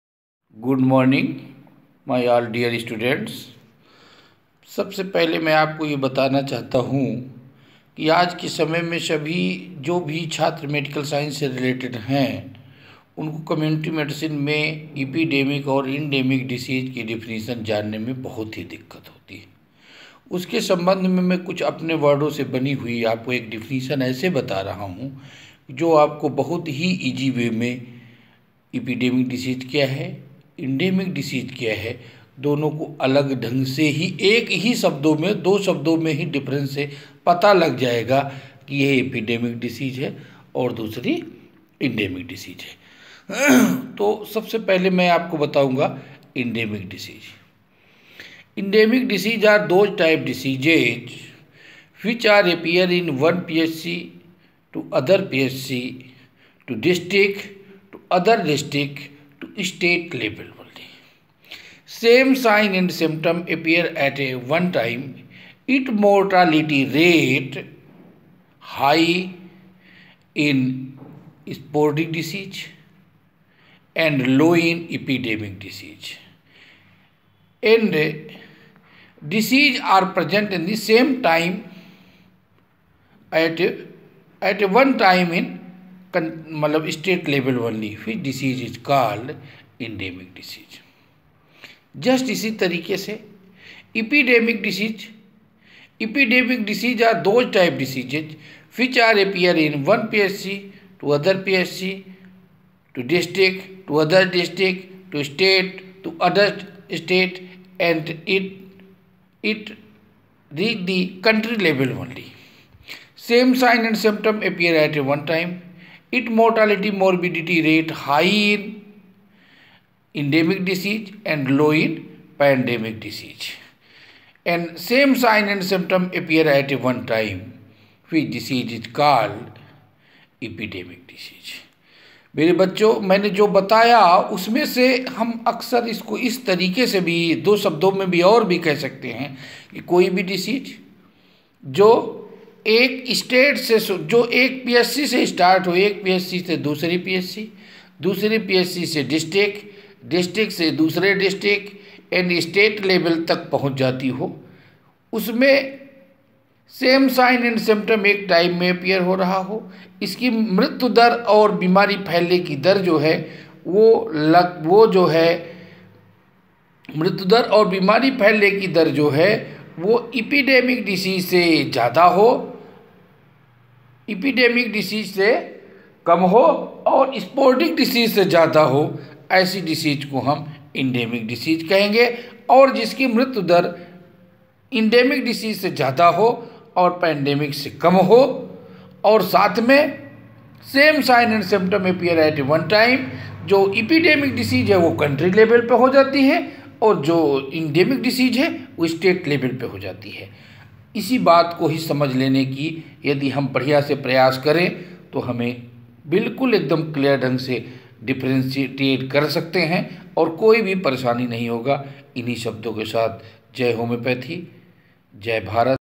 how to make best definition
how to make a perfect definition. define epidemic and pandemic disease